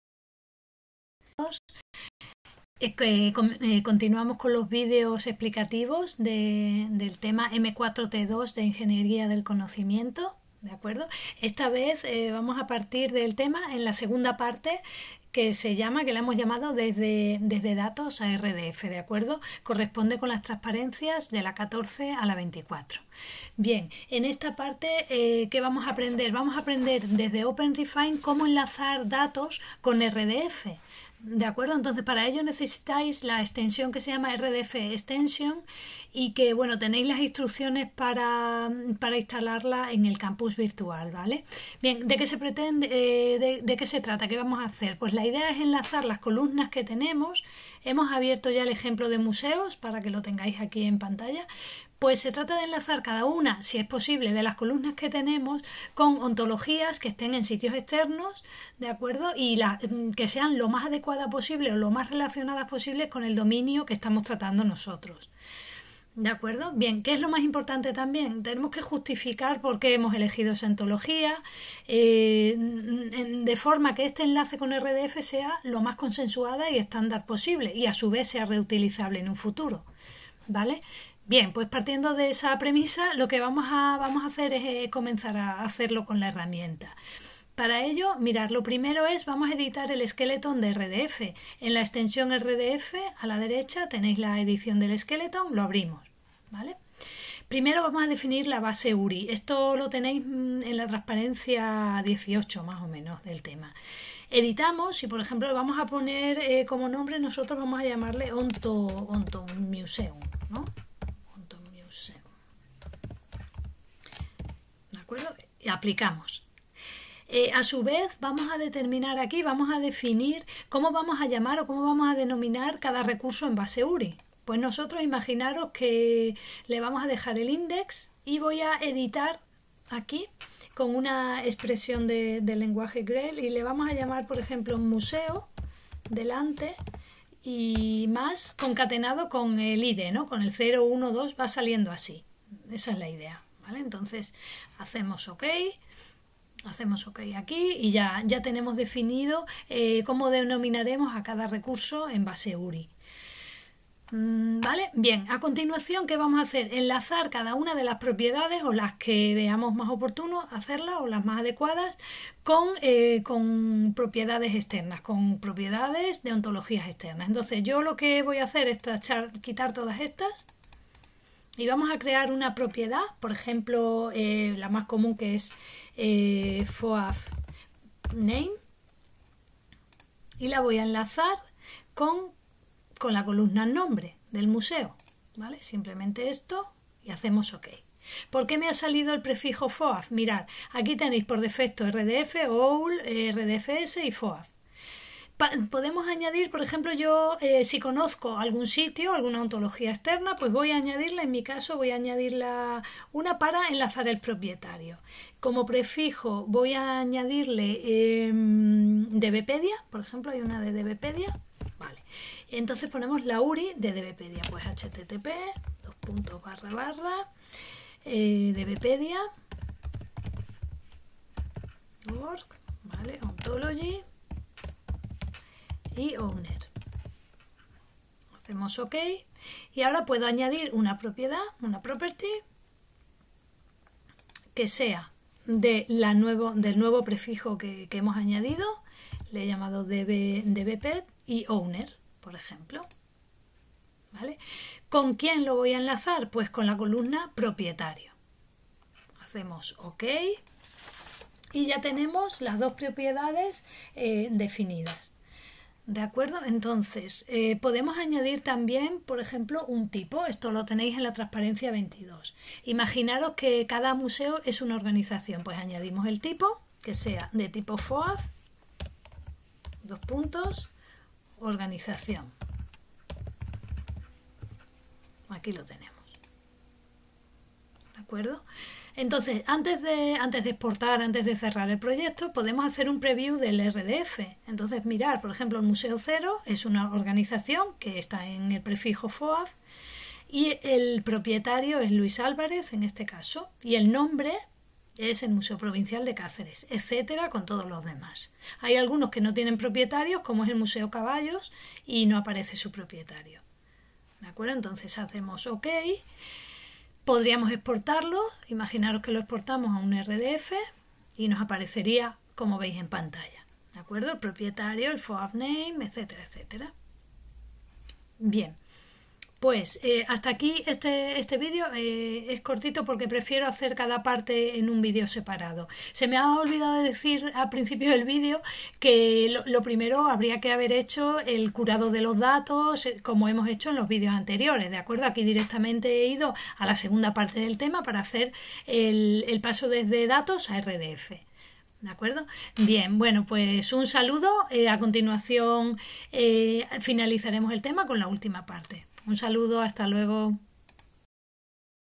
IC M4 T2 OpenRefine RDF vídeo3 (ejemplo museos)
Este vídeo explica la segunda parte (transparencias 14-24) del tema "IC_T4_M2_OpenRefine". En ellas, se toma como ejemplo el fichero "museosOrigen.xls".